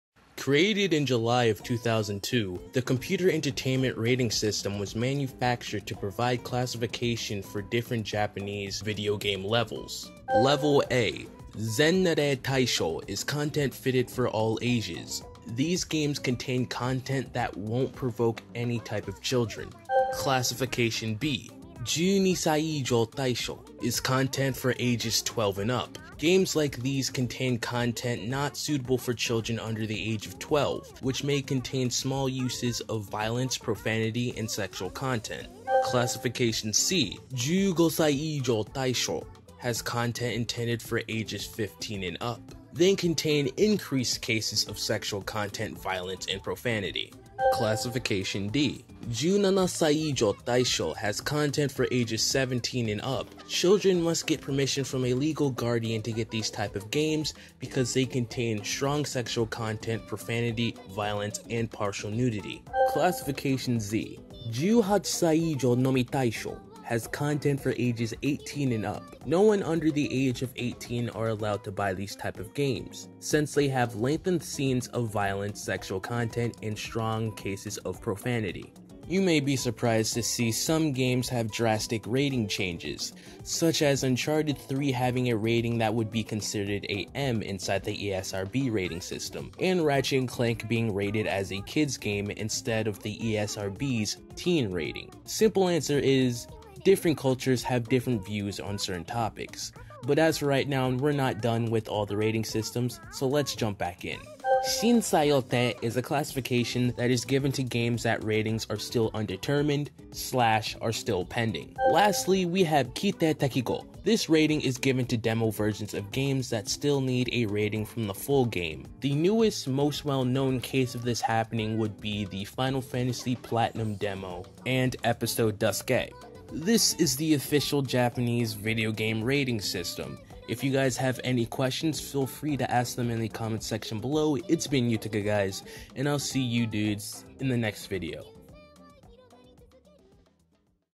Japanese Video Game Rating System Meanings
The meanings behind the computer entertainment rating organization.

Psn : Barmadia

Editor :Sony Vegas Pro / After Effects

Tumbnail Maker : PhotoShop

Console Recorder : HDPVR 1 / Elgato

Computer Recorder : Fraps